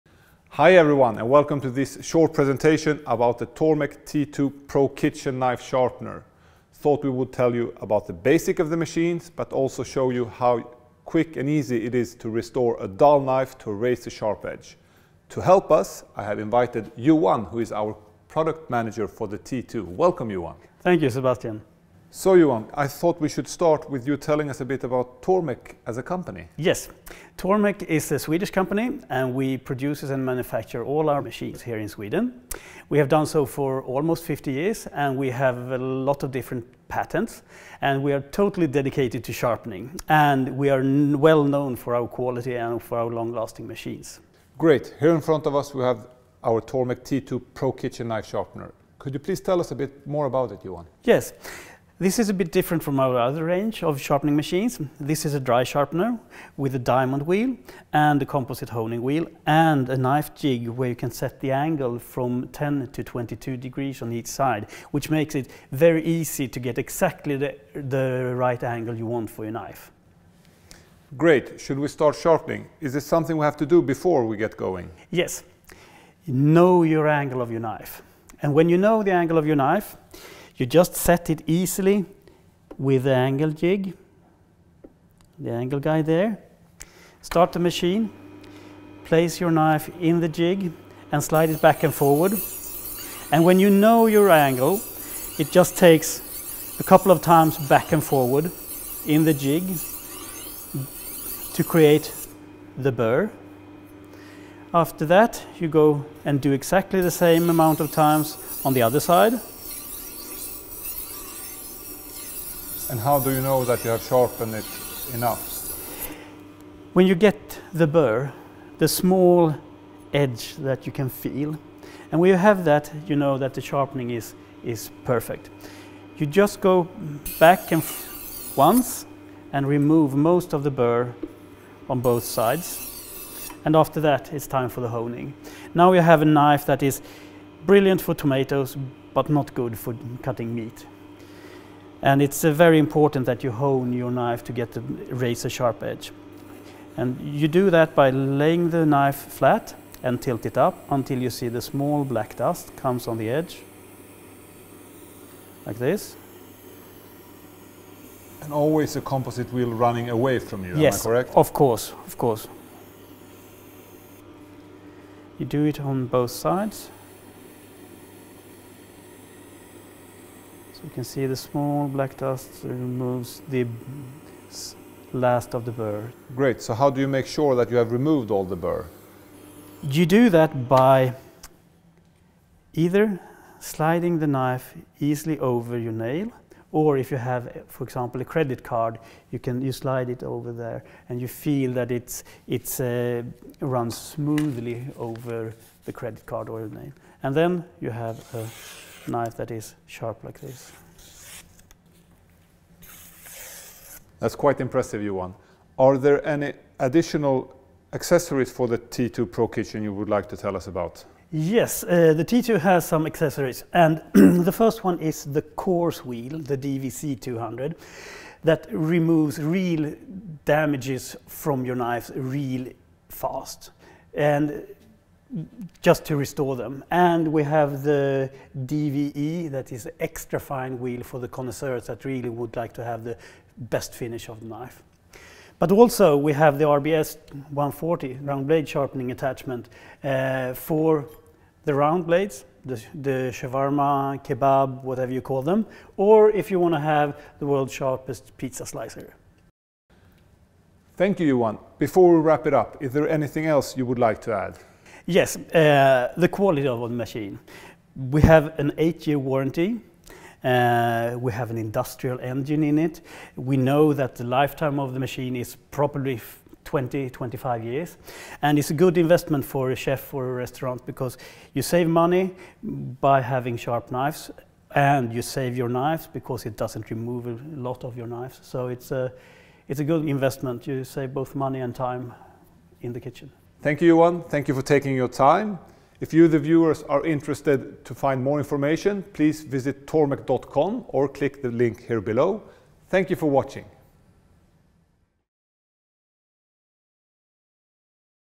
Quick Demo of the Tormek T-2 Pro Kitchen Knife Sharpener – English
The Tormek T-2 Pro Kitchen is specially designed to meet the professional’s demand for sharpness and user friendliness. You get sharp knives, just when you need it!

- Quick and easy set up.
- Professional sharpening.
- Always a constant angle.
- Diamond Wheel – for highest sharpness.
- Composite Wheel – for polishing.
- Full control of the knives.
- Speedy sharpening, one minute is enough.
- Silent running.

The fine-grained Diamond Wheel is optimized to provide a high level of sharpness and at the same time an efficient steel removal, without removing more steel than necessary. With the adjustable angle guide you can set the angle to suit the type of knife or usage. A larger edge angle gives a stronger, more sustainable edge. And the specially developed Composite Wheel with integrated polish removes the burr and polishes the bevel.